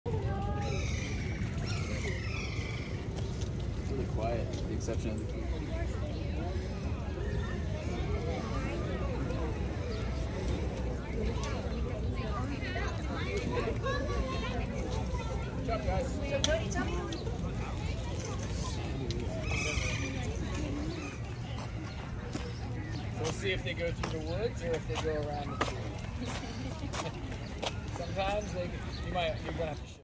PENGUIN PARADE
PENGUIN PARADE: Check out these king penguins marching their way through the Cincinnati Zoo & Botanical Garden!